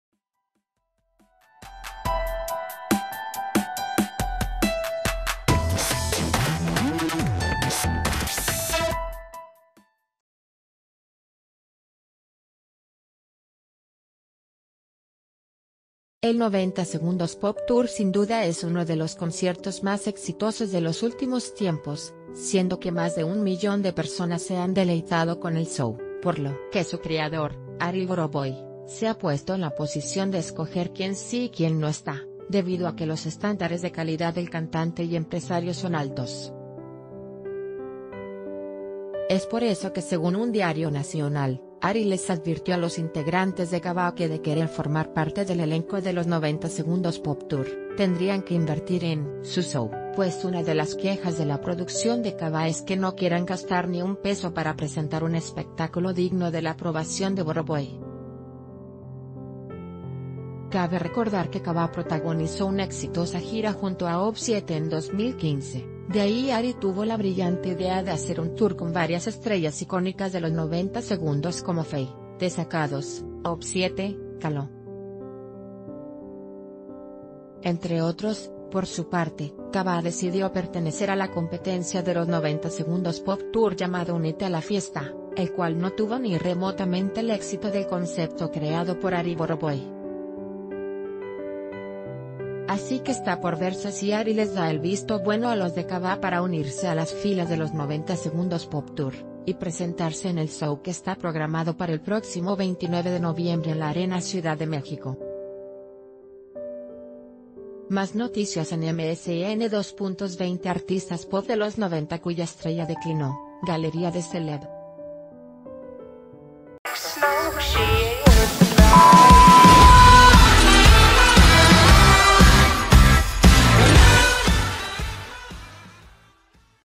Ari Borovoy se pone sus moños con los de Kabah para integrarlos al 90s Pop Tour
El 90s Pop Tour sin duda es uno de los conciertos más exitosos de los últimos tiempos, siendo que más de un millón de personas se han deleitado con el show, por lo que su creador, Ari Borovoy, se ha puesto en la posición de escoger quién sí y quién no está, debido a que los estándares de calidad del...